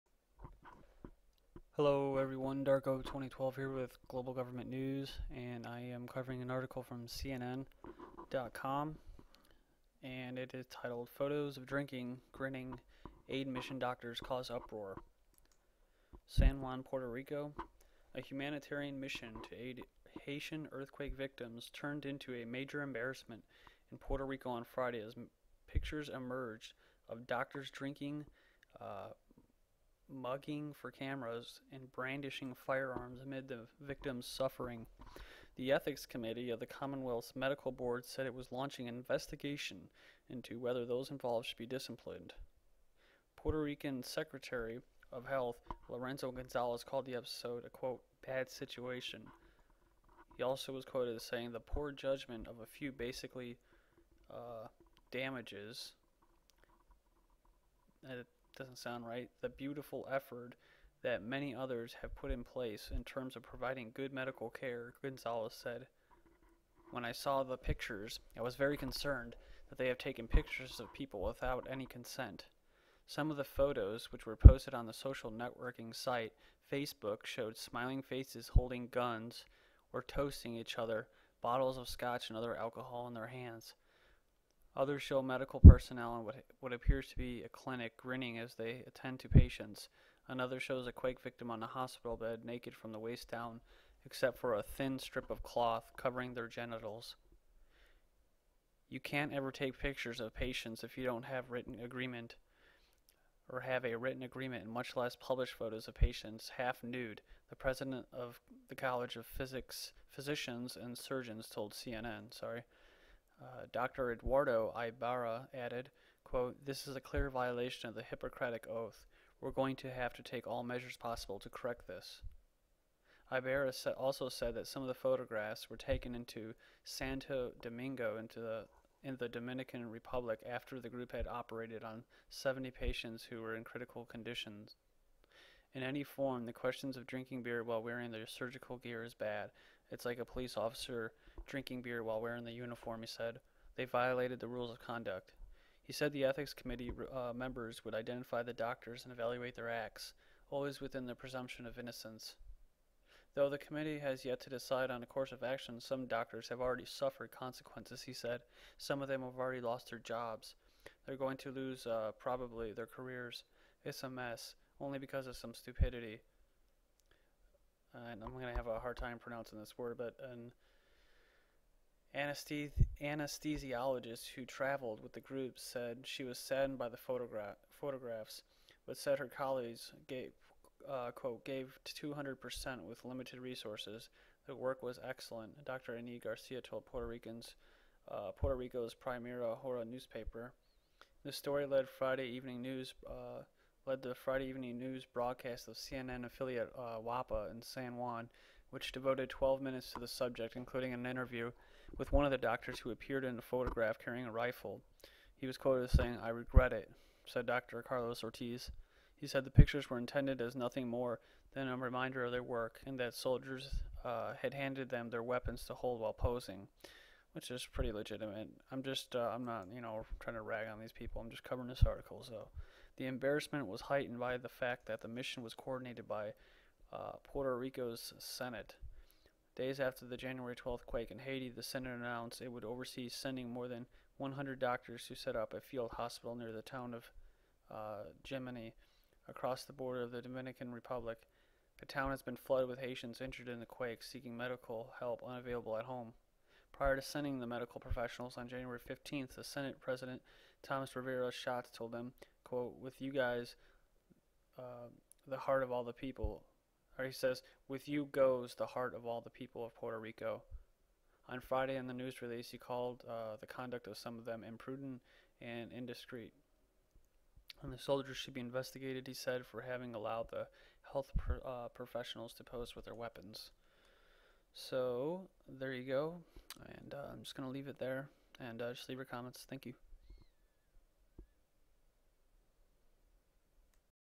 GGN- Photos of Drinking, Grinning Aid Mission Doctors Cause Uproar
San Juan, Puerto Rico (CNN) -- A humanitarian mission to aid Haitian earthquake victims turned into a major embarrassment in Puerto Rico on Friday as pictures emerged of doctors drinking, mugging for cameras and brandishing firearms amid the victims' suffering.

The ethics committee of the commonwealth's medical board said it was launching an investigation into whether those involved should be disciplined.

Puerto Rican Secretary of Health Lorenzo Gonzalez called the episode "a sad situation."

"The poor judgment of a few basically damages the beautiful effort that many others have put in place in terms of providing good medical care," Gonzalez said. "When I saw the pictures, I was very concerned that they have taken pictures of people without any consent."

Some of the photos, which were posted on the social networking site Facebook, show smiling doctors holding guns or toasting each other, bottles of scotch or other alcohol in their hands. Others show medical personnel in what appears to be a clinic, grinning as they attend to patients. Another shows a quake victim on a hospital bed, naked from the waist down except for a thin strip of cloth covering the genitals.

"You can't ever take pictures of patients if you don't have a written agreement and much less publish photos of patients half-nude," the president of the College of Physicians and Surgeons told CNN. Dr. Eduardo Ibarra added, "This is a clear violation of the Hippocratic Oath ... We're going to take all the measures possible to correct this."